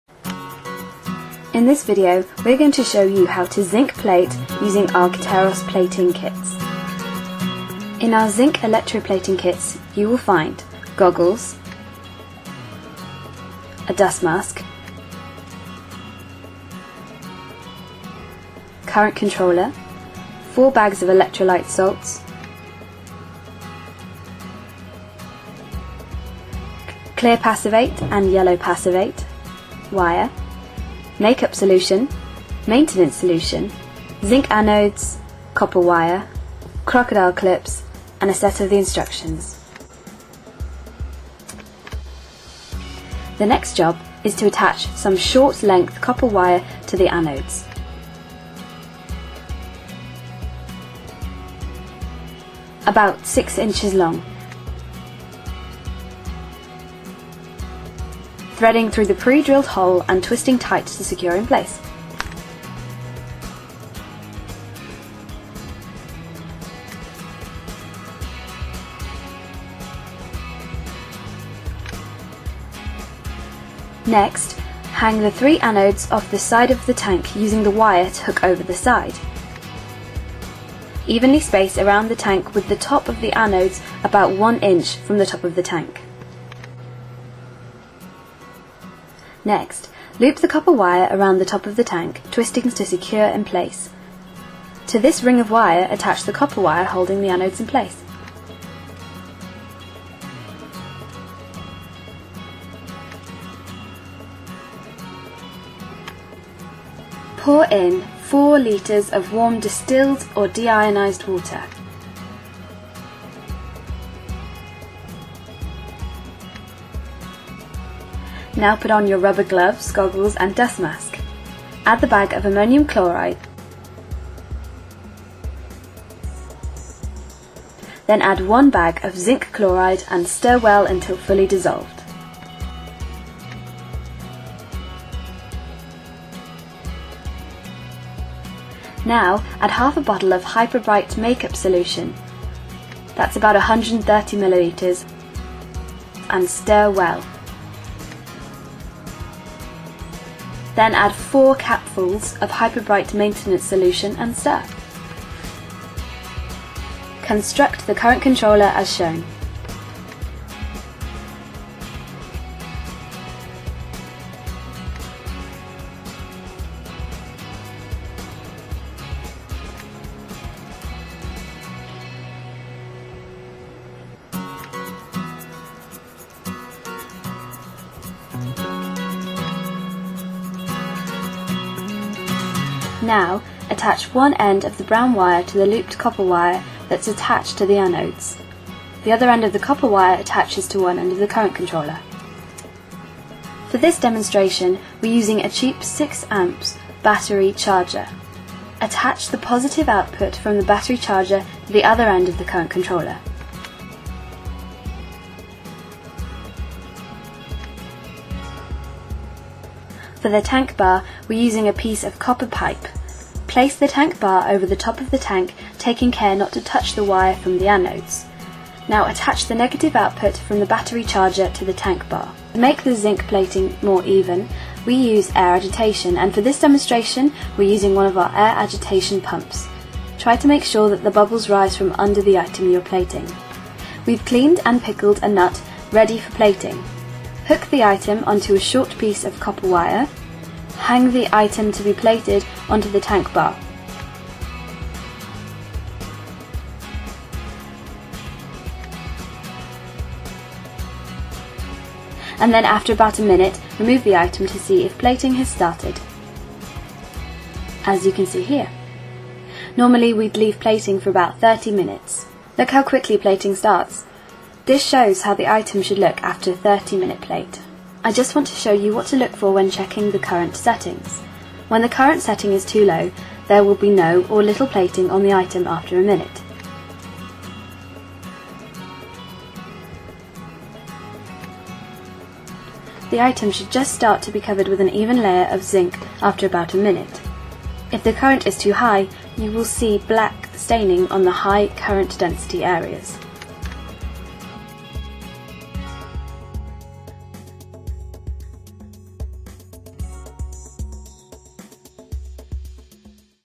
Gateros Plating - Zinc
In this video, you will see just how easy it is to electro-plate using Gateros Plating, Zinc tank plating kits.